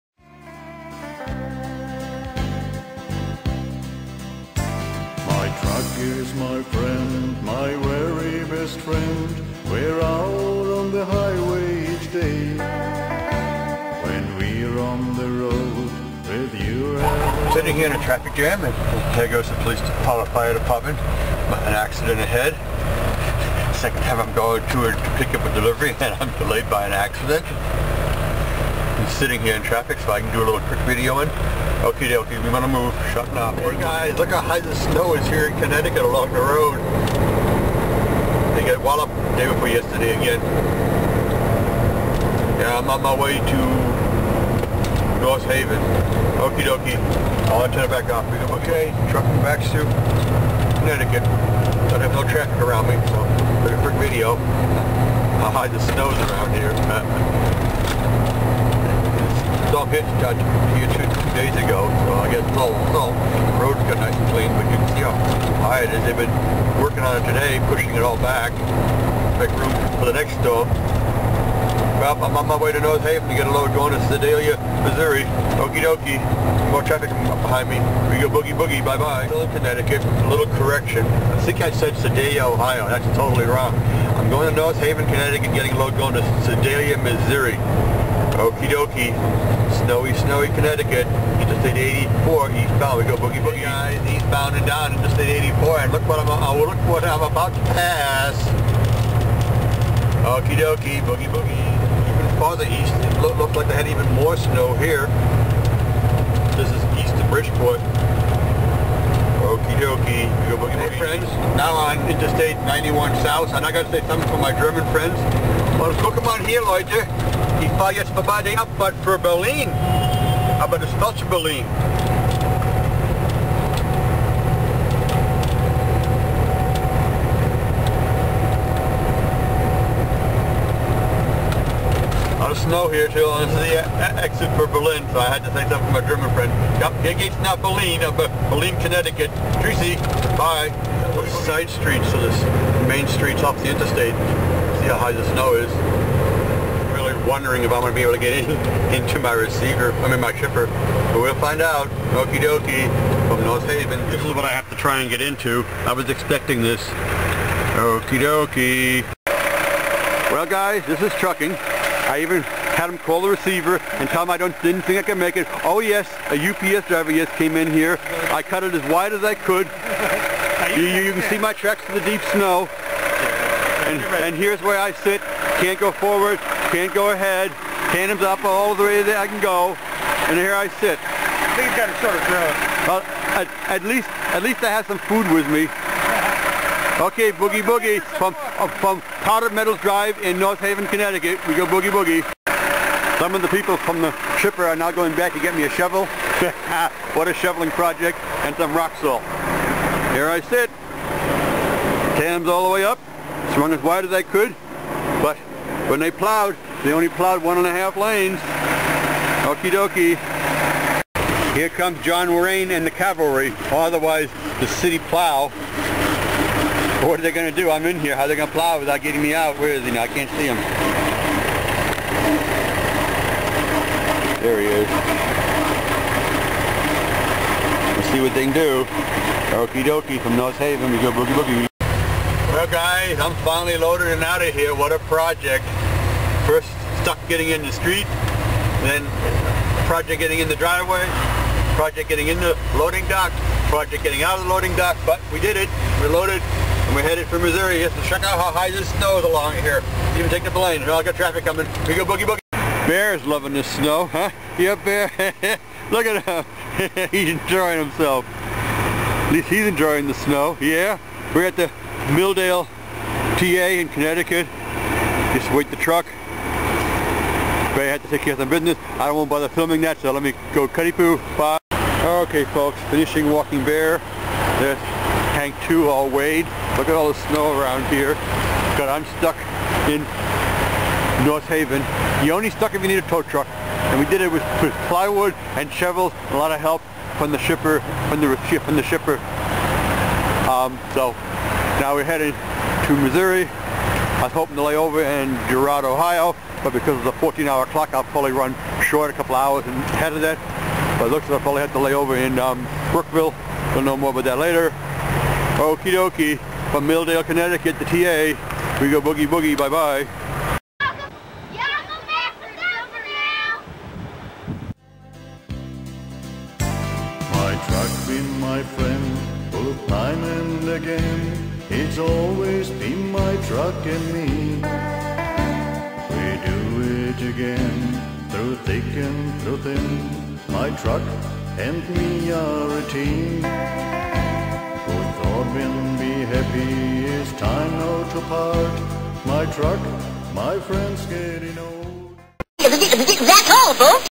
Almost Stuck in North Haven CT 1-28-11.wmv
Going to pick up a load at North Haven, CT 2 days after a major snow storm